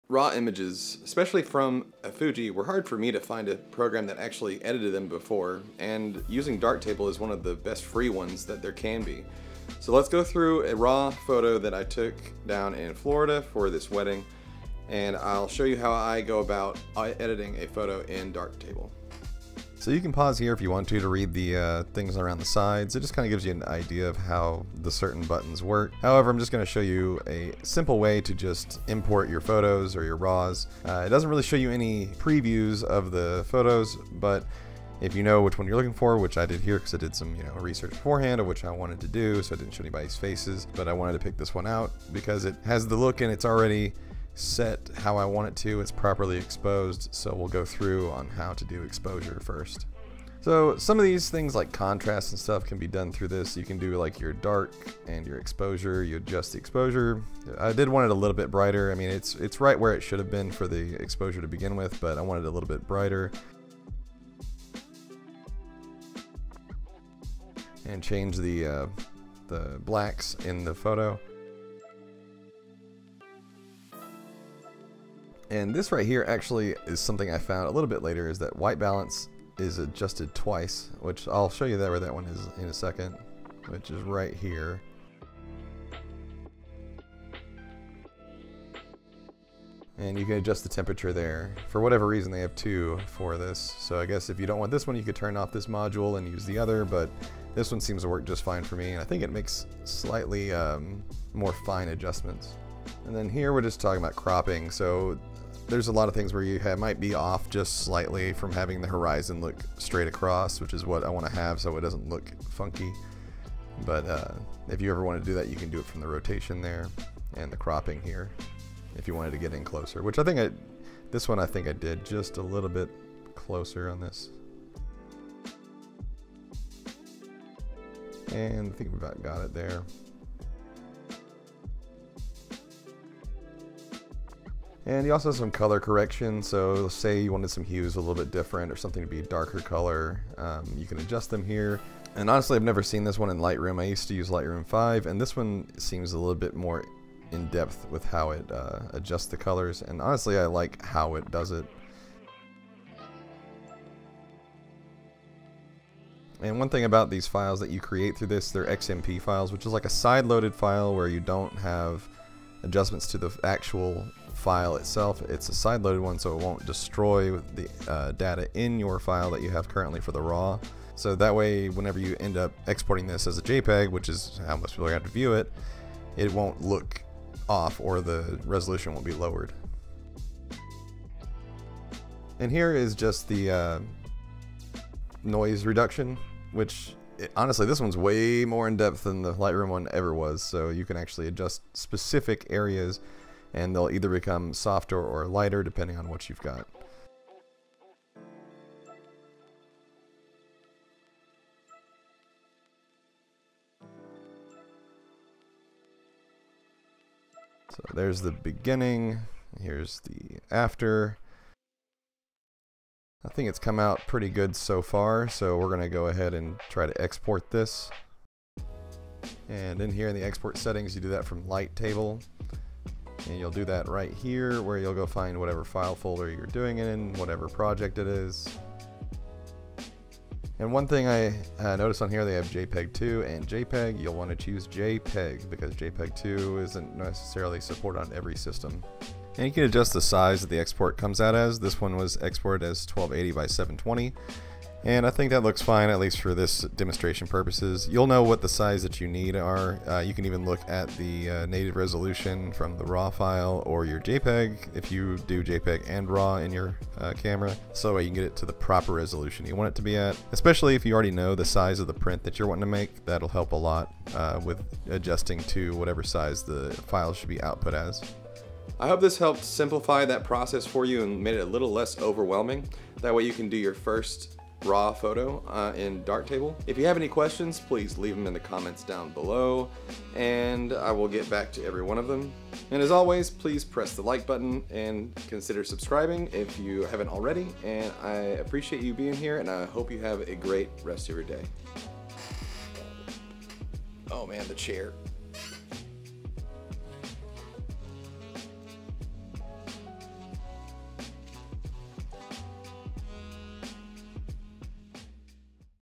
Darktable Tutorial for Beginners – Free Lightroom Alternative!
Looking for a free alternative to Lightroom? In this Darktable tutorial for beginners, I’ll walk you through everything you need to know to start editing RAW photos—completely free! From importing images to basic adjustments like exposure, white balance, and exporting, this video gives you a full walkthrough of the Darktable photo editing workflow.

Whether you're a budget-conscious photographer or just getting into RAW editing, this video will help you get started quickly with Darktable.

Chapters:
00:00 – Intro
00:20 – Importing Images
00:55 – Exposure
01:13 – Temperature/White Balance
01:39 – Rotation and Cropping
02:14 – Color Correction
03:02 – Denoise
03:23 – Before and After
03:38 – Exporting
04:29 – Wrap-up